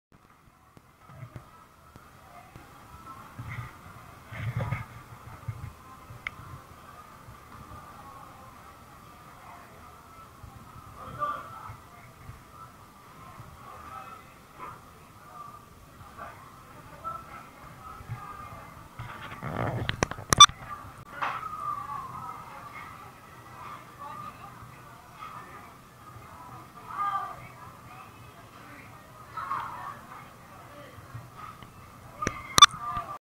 SF Boudin Bakery and Restaurant
Famous for its sourdough bread. Mum buying croissants and Pain au Chocolat for our breakfast